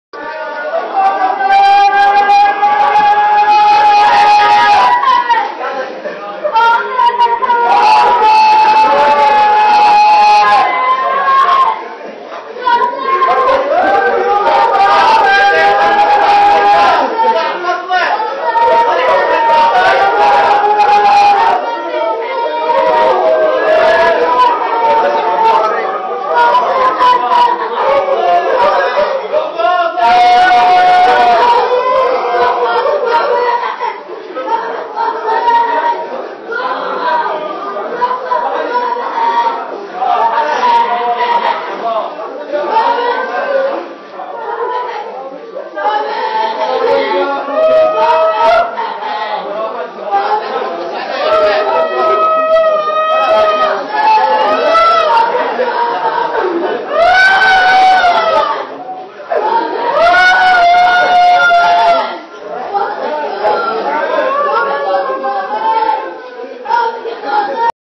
الدراز - توديع الشهيد فاضل العبيدي من داخل المغتسل 10-3
الدراز - توديع الشهيد فاضل العبيدي من داخل المغتسل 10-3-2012